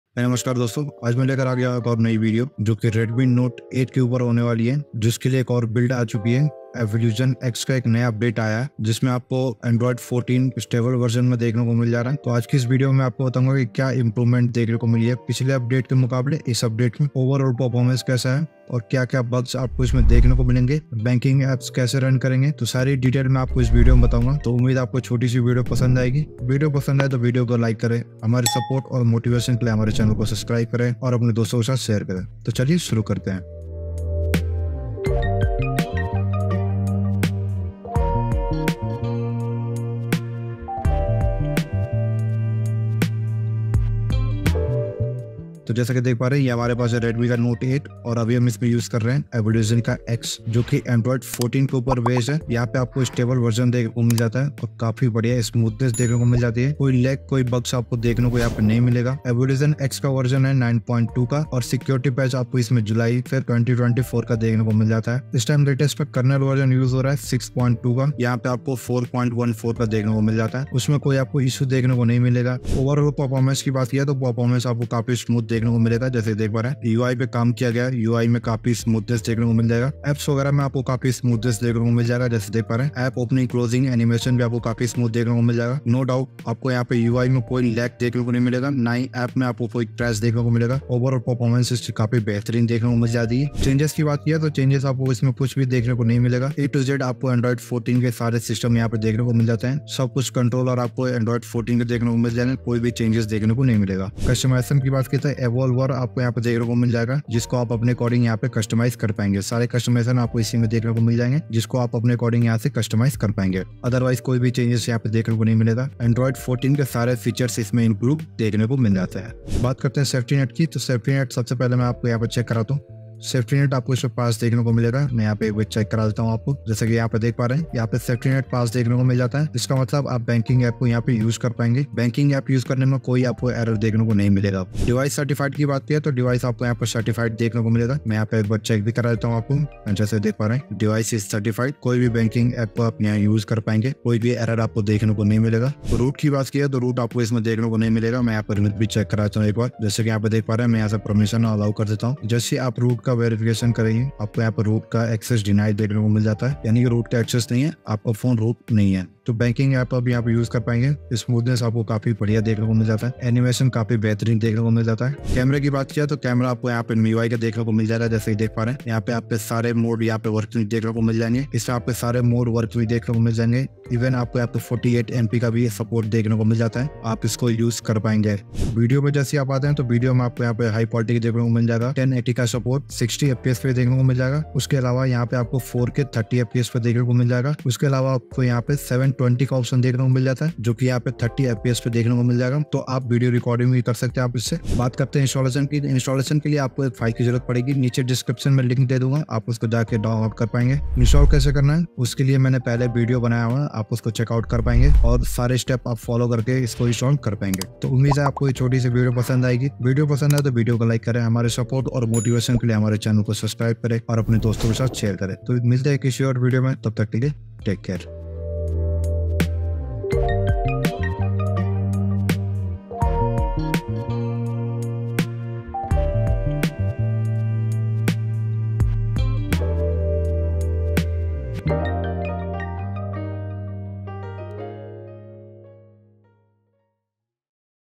Evolution X Redmi Note 8 Review: The Ultimate Custom ROM?
In this YouTube video, we delve into the Evolution X custom ROM for the Redmi Note 8, exploring its features, performance, and overall user experience. Is Evolution X the ultimate custom ROM for the Redmi Note 8? Watch to find out!